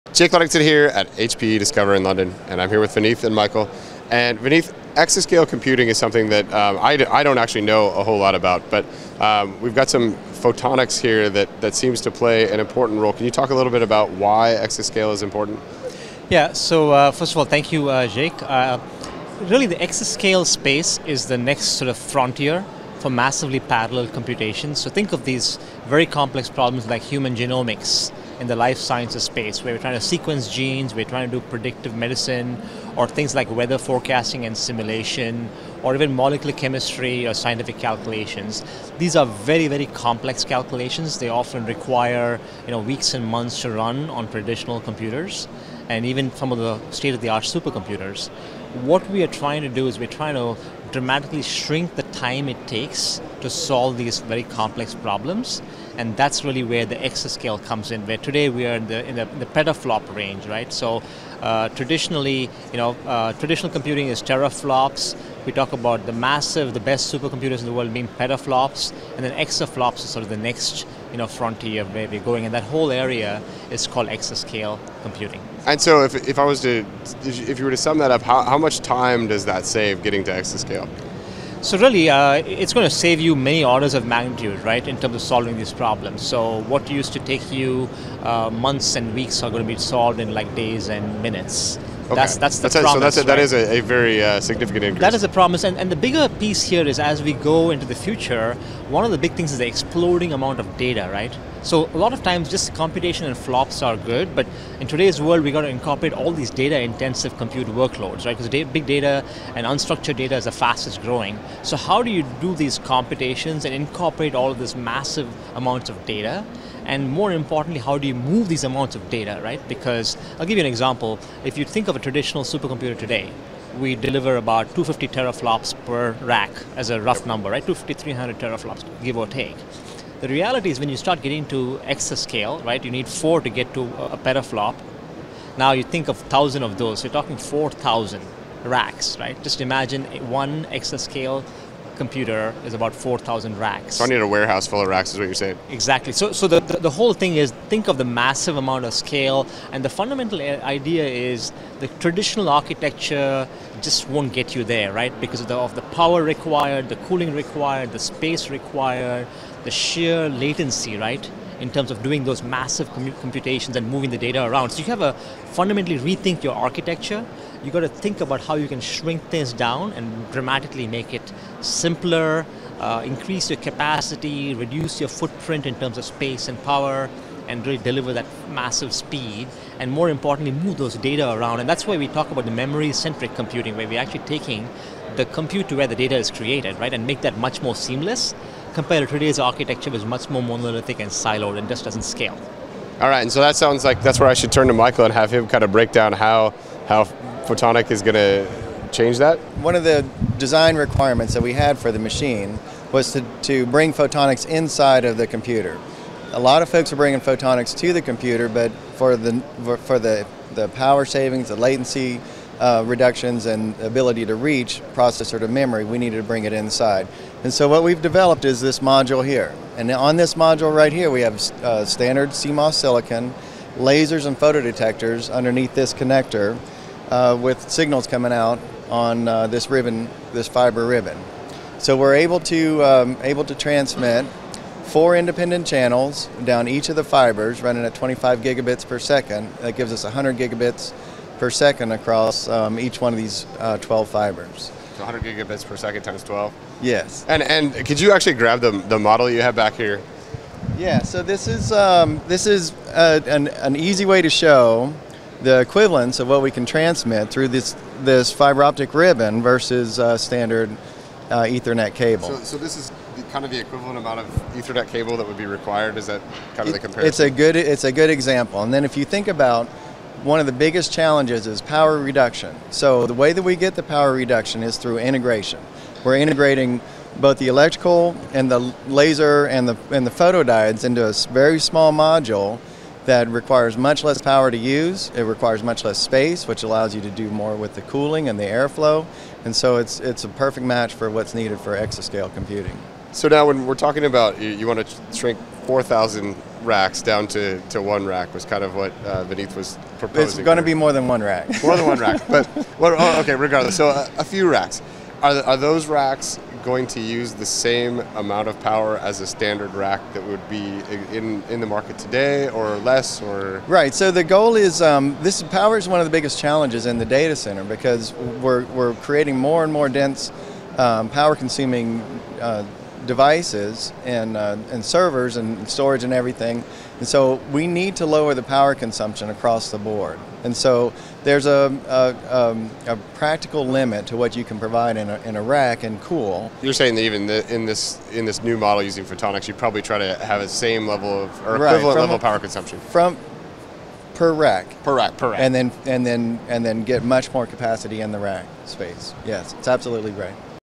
Photonics Accelerates the Journey to Exascale Computing
As we continue to gather more and more data from sensors, the compute power required to process that data is also growing. Keeping up with the data processing either requires an ever growing data center footprint or it requires rethinking the way computers are built. Photonics is one of the technologies that is making this new model of computing possible by allowing more data to be moved more quickly with a vastly reduced footprint. At HPE Discover in London, Vineeth Ram and Michael McBride explain how Photonics is helping drive the path to exascale computing.